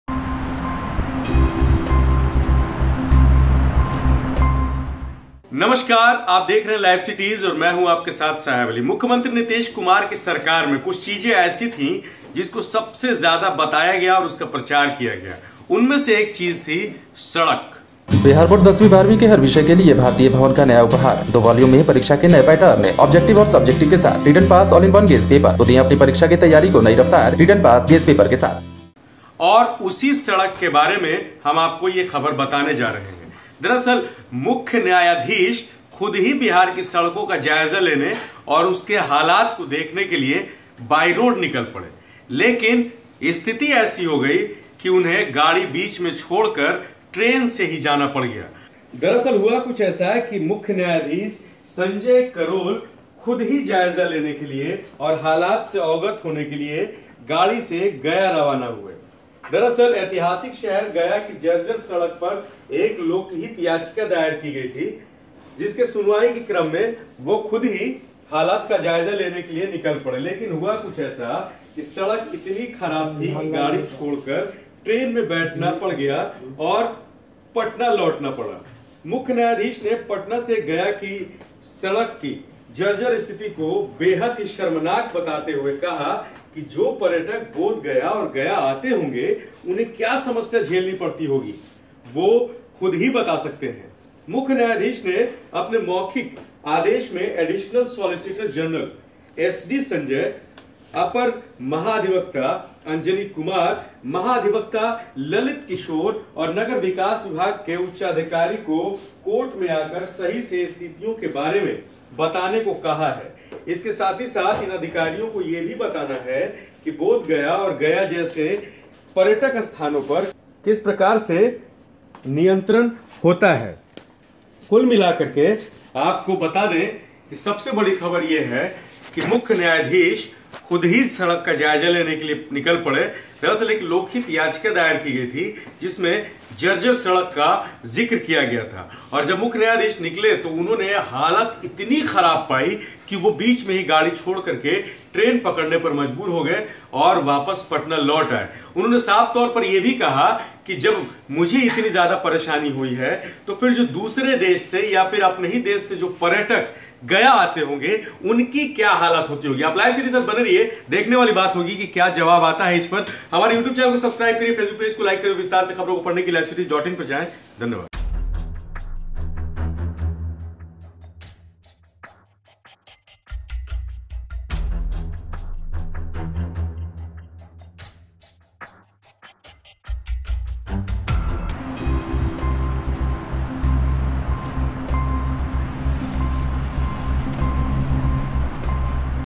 Chief Justice Sanjay Karol क्यों सड़क छोड़ Train से Patna लौटने को हुए मजबूर, जा रहे थे गया
Chief Justice Sanjay Karol क्यों सड़क छोड़ Train से Patna लौटने को हुए मजबूर, जा रहे थे गया | LiveCities